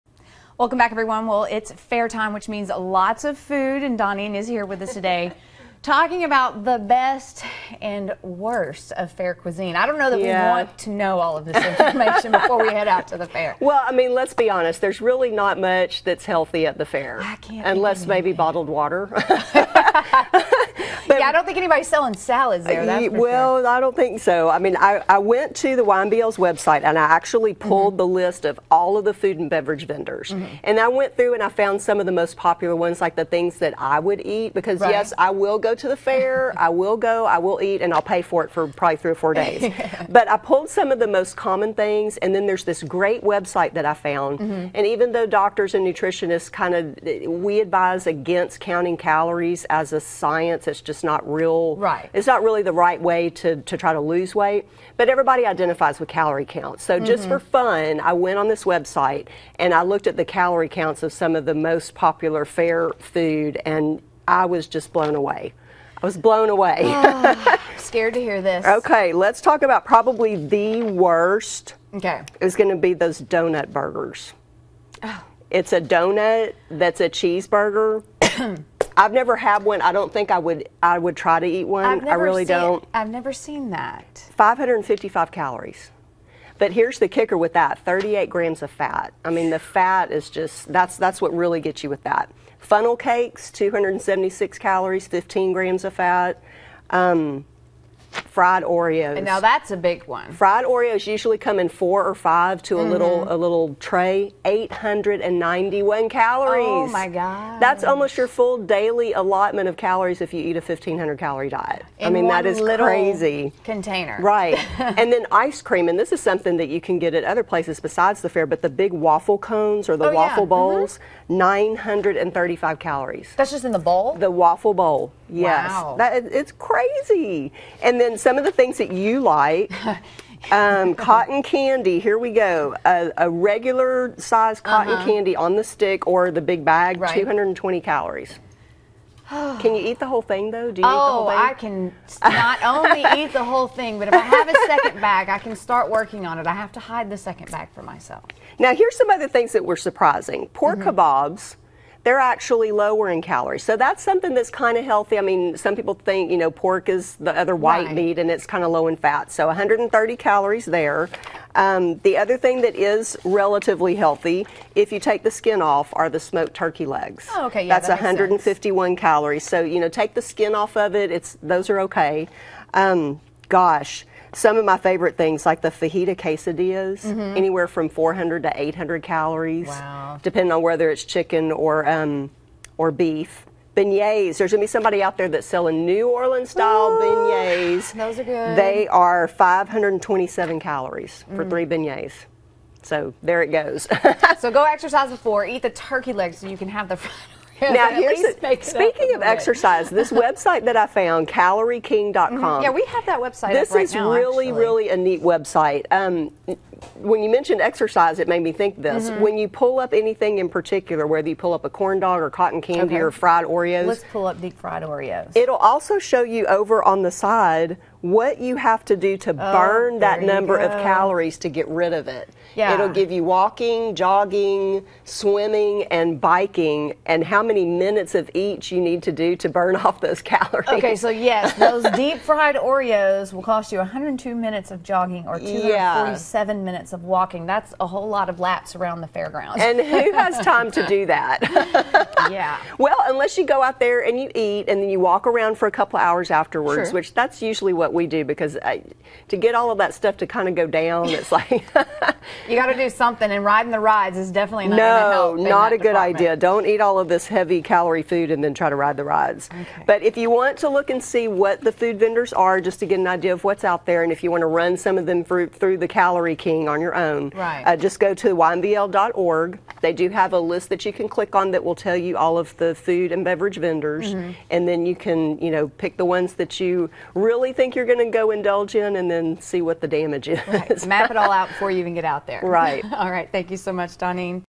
Doneane discusses fair cuisine
Doneane discusses the best and worst fair cuisine.